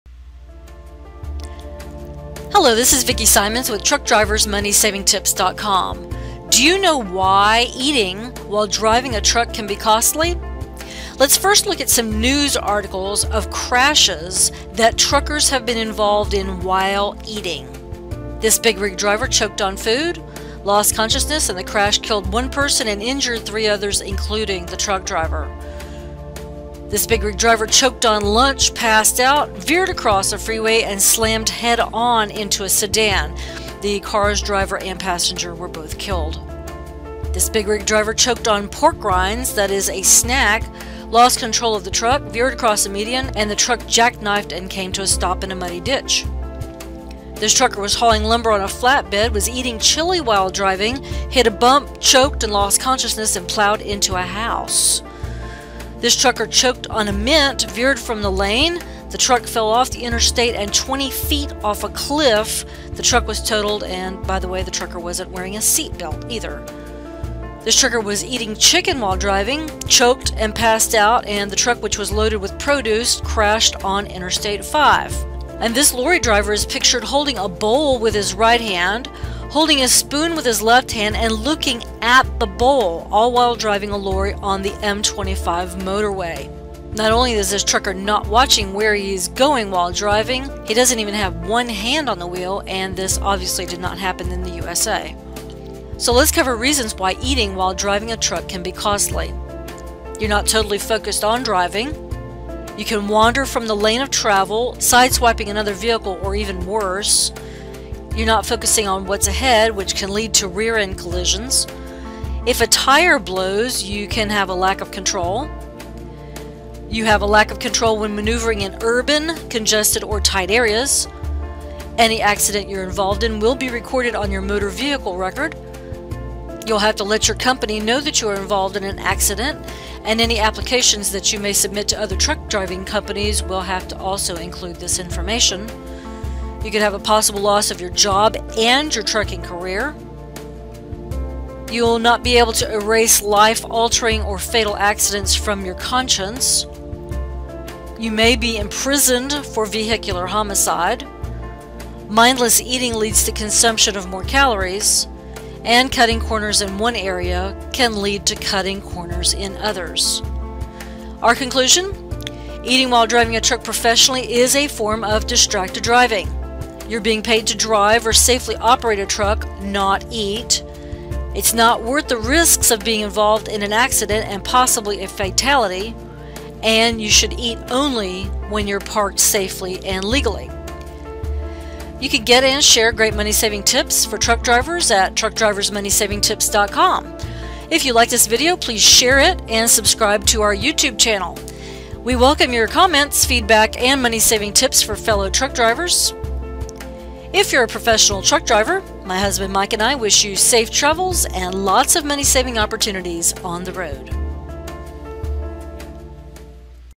Truck Drivers, Eating While Driving, and Costs
Do you know the reasons why eating while driving a truck can be costly?

We review some truck accident articles (with some truck accident pictures) about truckers who crashed because of -- or were caught on camera -- eating while driving a truck.

A trucker may possibly get a ticket for eating while driving, but big rig wrecks always cost money. Sometimes these semi truck accidents turn into deadly big rig crashes!

Do trucking companies -- or other training for truck driving jobs -- address this issue directly? When you're becoming a truck driver -- that is, a professional truck driver on the road -- you must learn to safely operate the truck. The evidence is clear: A trucker who eats while driving cannot steer the truck as well as when using both hands.

Don't be filmed or otherwise caught on dashcam eating while driving a commercial motor vehicle. Eat healthy meals, yes, but park safely and legally first.